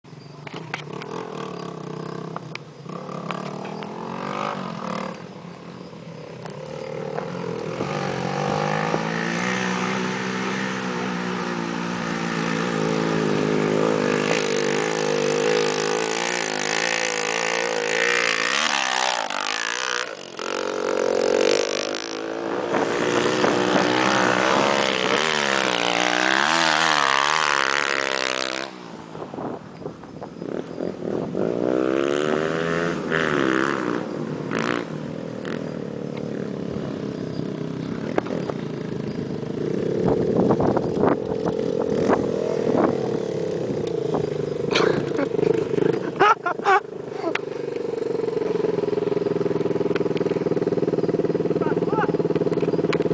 Trilha com subidão em Brusque subidão.
subida em terraplanagem no bairro planalto, sahara, cg 125 e xlr 125...